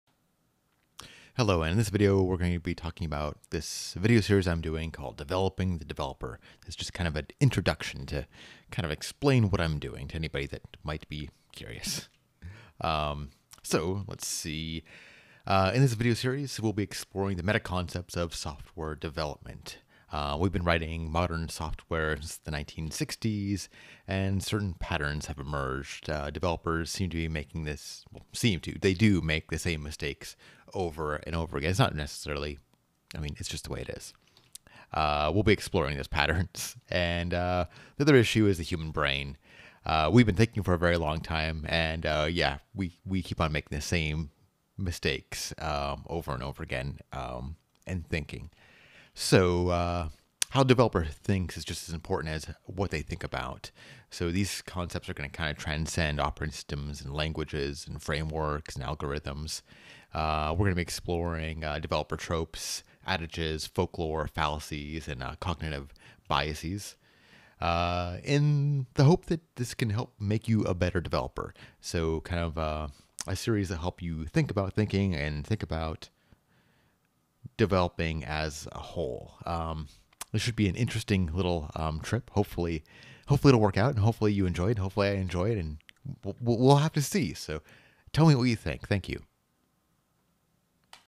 Developing Developers
In this series of videos, we will explore the meta concepts of software development. We have been writing modern software since the 1960's.  Developers seem to make the same mistakes over and over again. And certain patterns have emerged. We will be exploring these patterns. Another issue is the human brain. Humans have been thinking for a very long time. Yet, humans still make the same mistakes in thinking over and over again. How a developer thinks is just as important as what they think about. These concepts transcend OSs, languages, frameworks, and algorithms. We is be exploring developer tropes, adages, folklore, fallacies, and cognitive biases. I hope that these concepts will help you become a better developer.